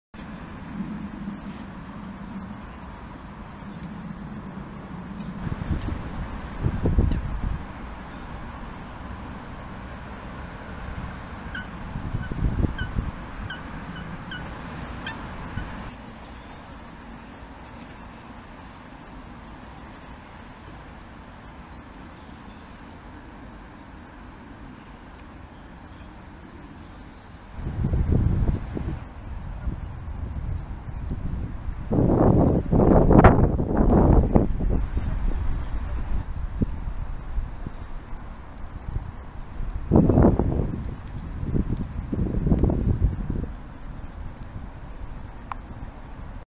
Stanley Park lake at Night,
Stanley Park Lake.. more of an early evening video than a night video really :)

The lake is so peaceful at this hour.